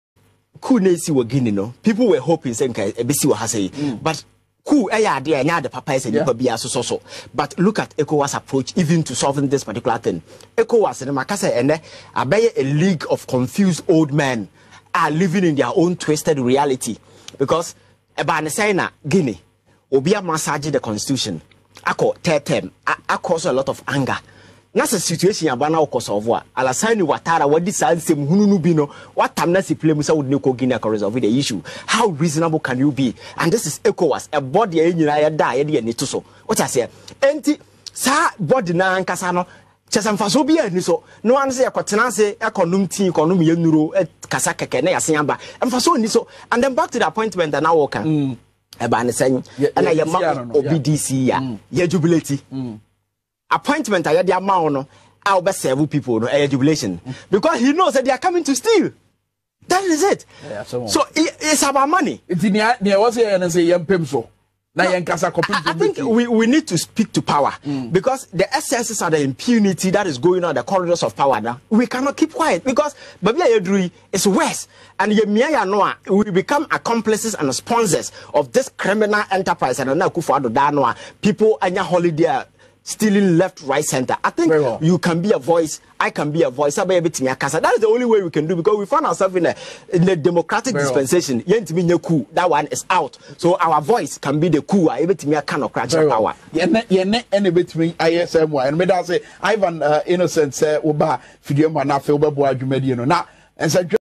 Youth Activist Ivan Kyei "Harshly" calls out ECOWAS leaders over the instability in Guinea.
Youth Activist Ivan Kyei "Harshly" calls out  ECOWAS leaders over the instability in Guinea; says the Regional Bloc is useless